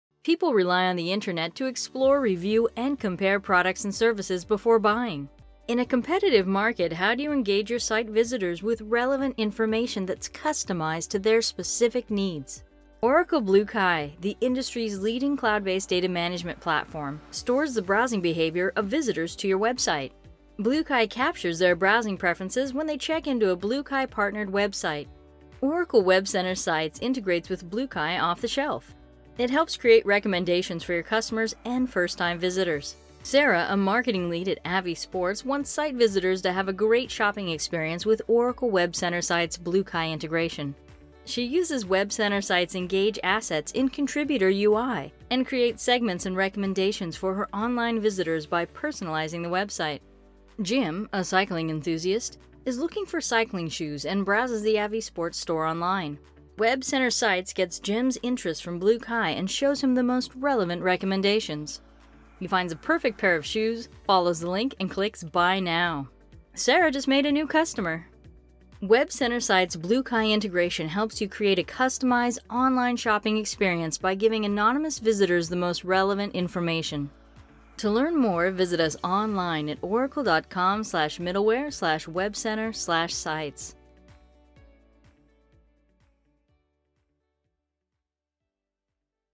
Integration of WebCenter Sites with Bluekai
Bluekai helps you tap into the numerous anonymous visitors who visit your site. Enabling Bluekai integration with WebCenter Sites allows you to customize your website with online interests of site visitors, including ones who are anonymous.
1080p/720p HD.